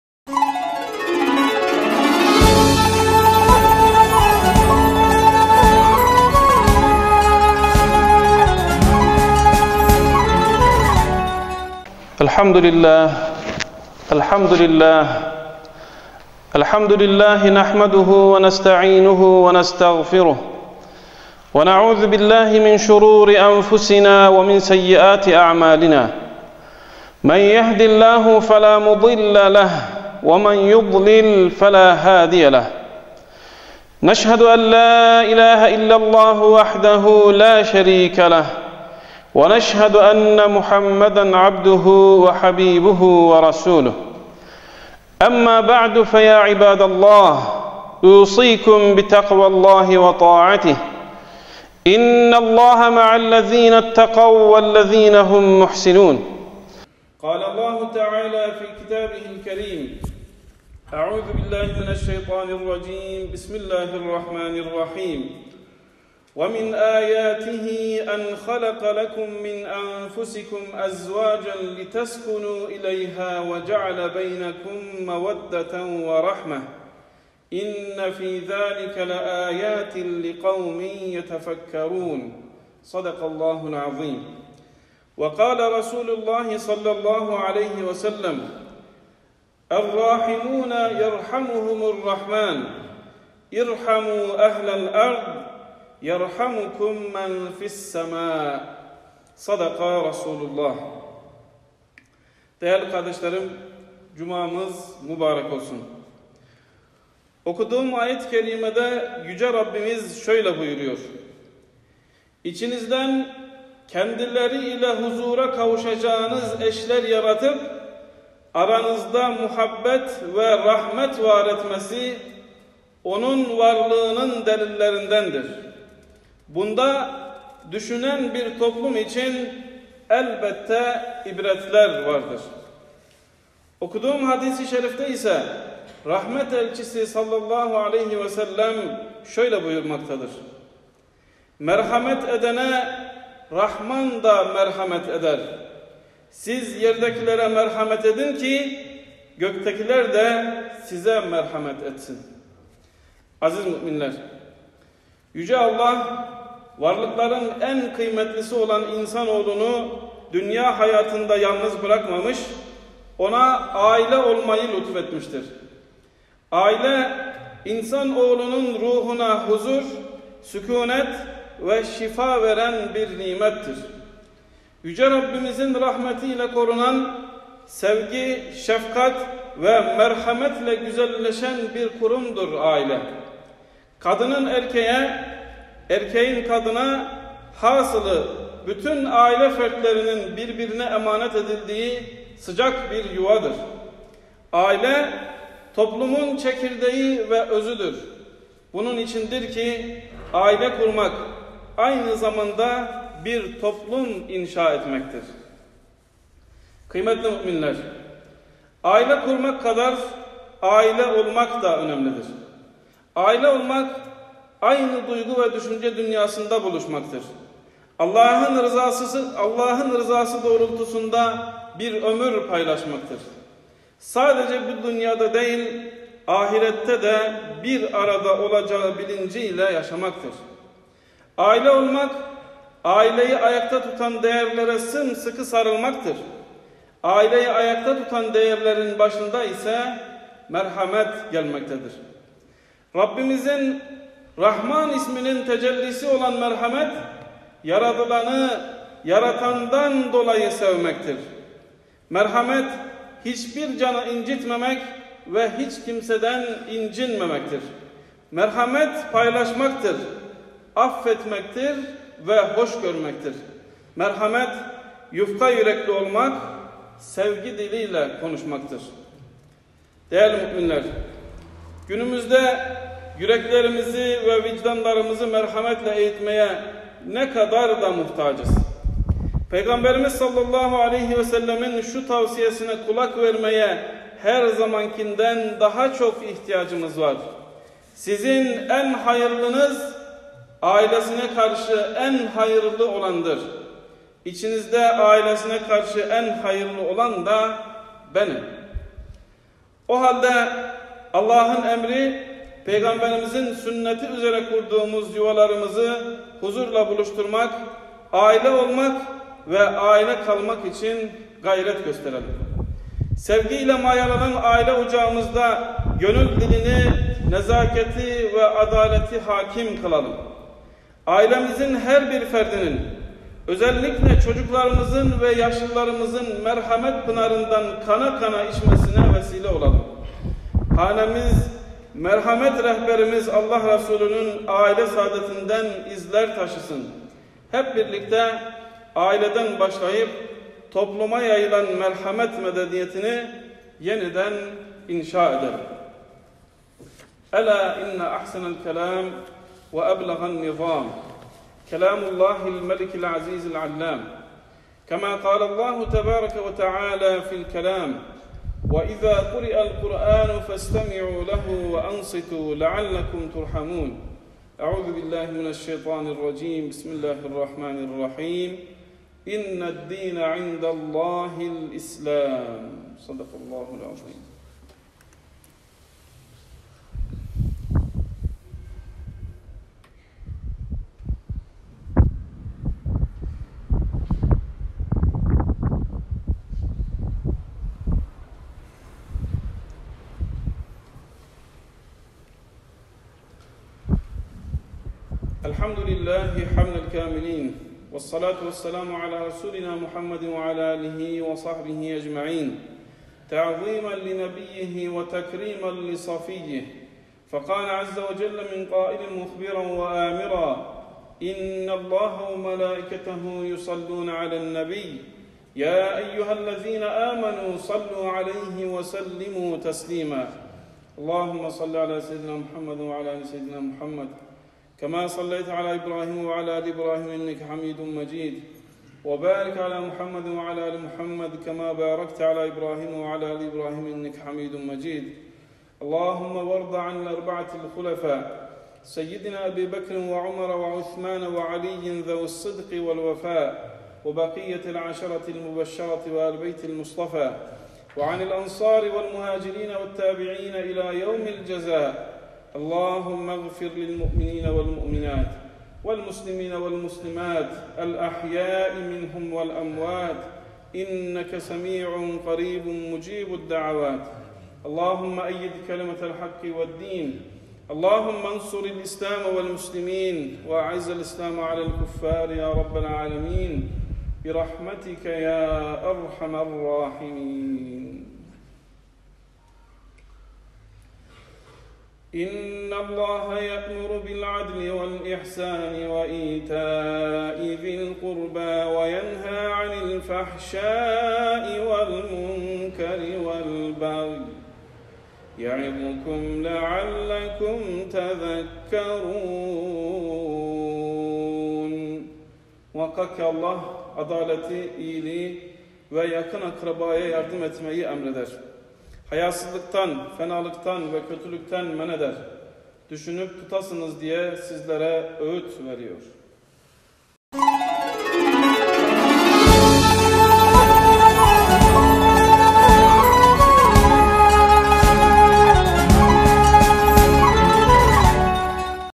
AİLEMDE MERHAMET VE HUZUR VAR
CUMA HUTBESİ : AİLEMDE MERHAMET VE HUZUR VAR

ŞEYH MUHAMMED ELFARUKİ CAMİİ

İMAM HATİP:MESUT KAYA